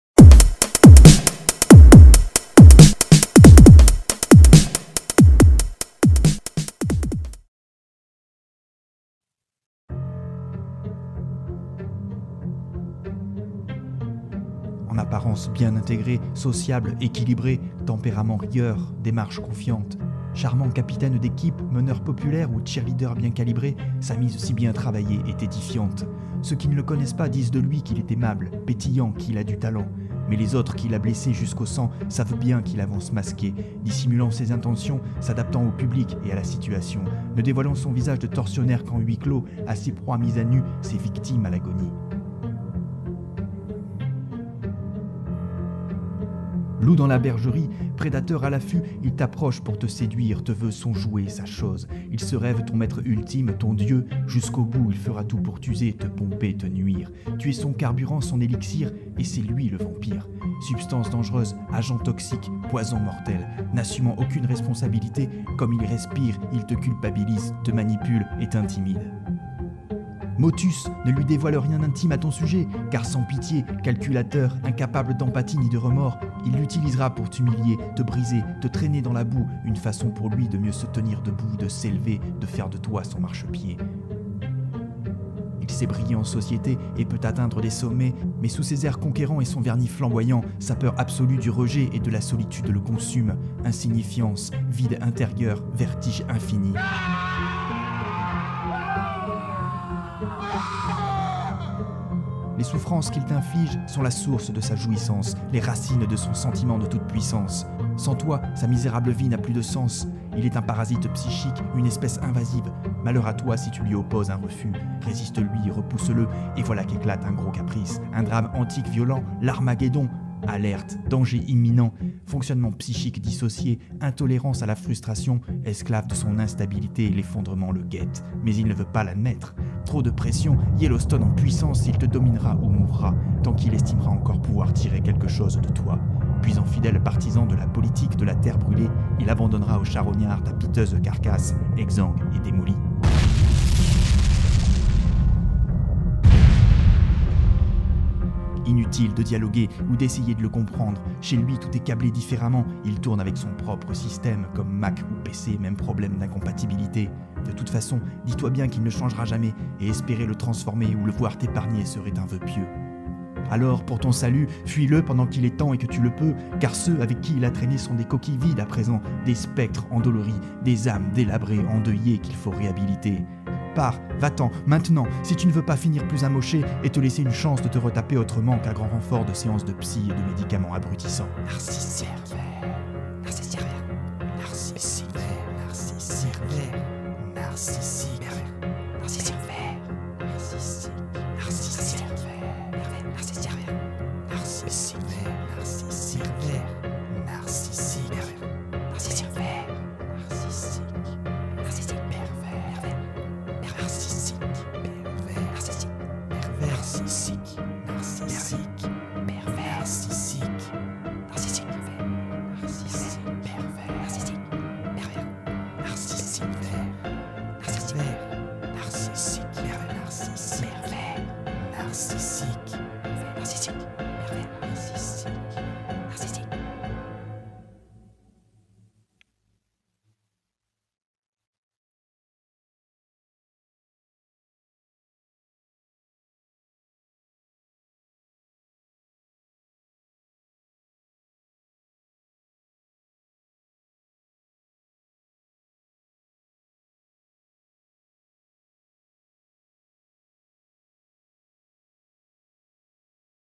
Slam Time#5 : Menace PN
Minute psychiatrique. Toute ressemblance avec des personnes existant ou ayant existé ne serait pas purement fortuite.

Un texte écrit et lu par Romano Vlad Janulewicz.

Crédits photographiques : Pixabay